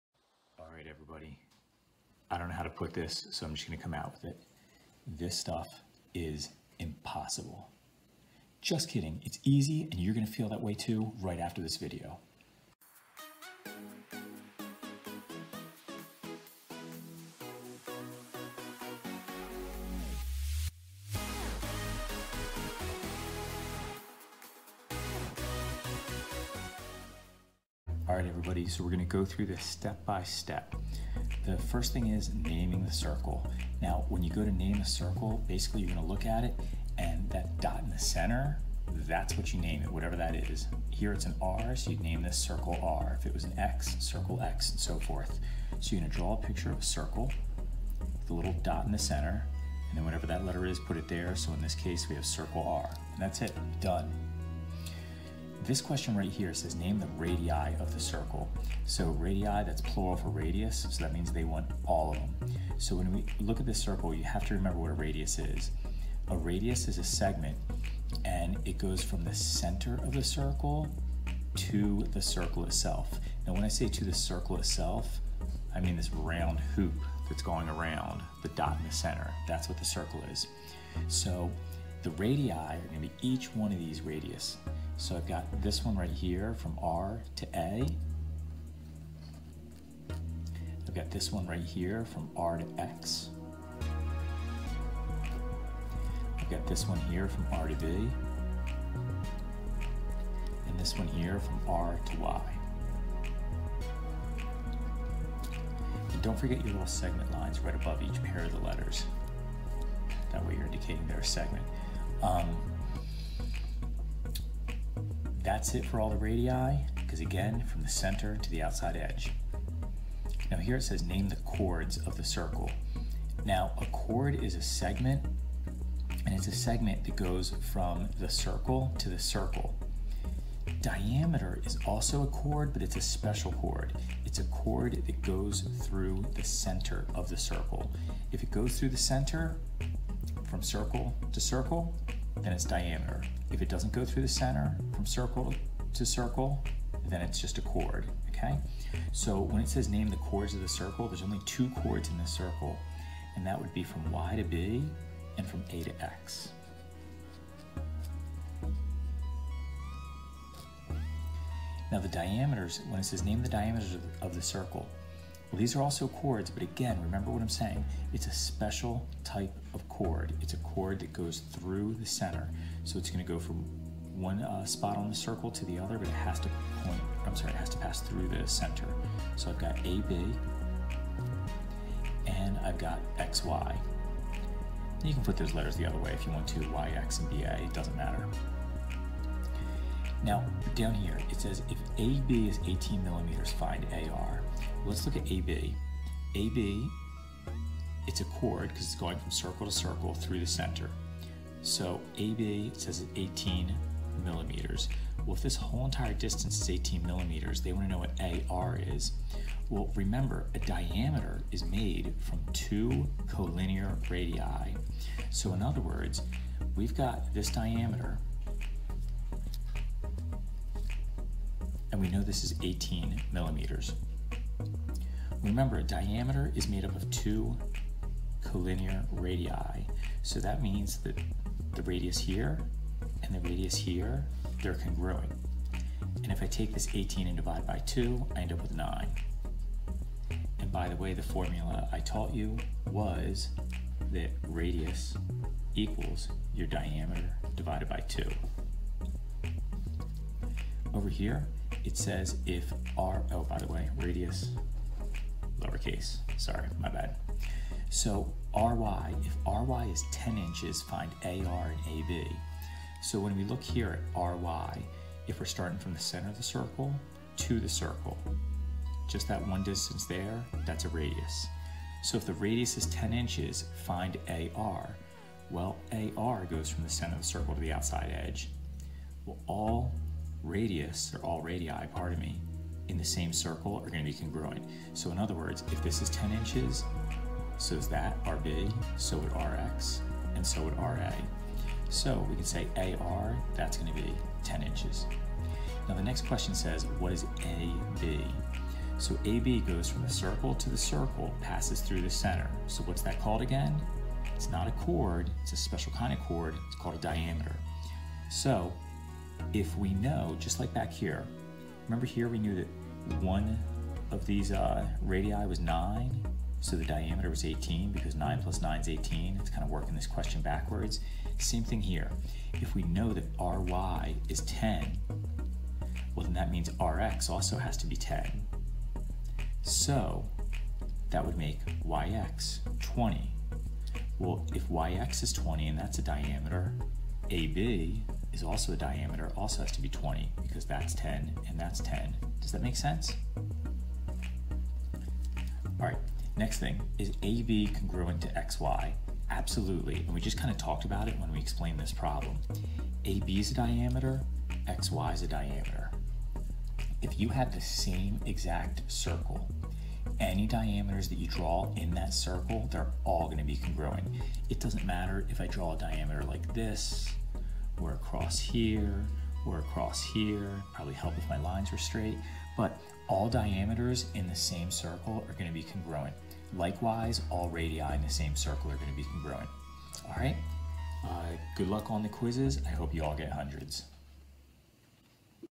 Geometry - Circles and Segments! Review
In this video we review the practice problems on circles, radii, chords, and diameters.

We also discuss the relationship between radii and diameter of a circle.

In the text, this is Chapter 10 Section 1, parts 1 and 2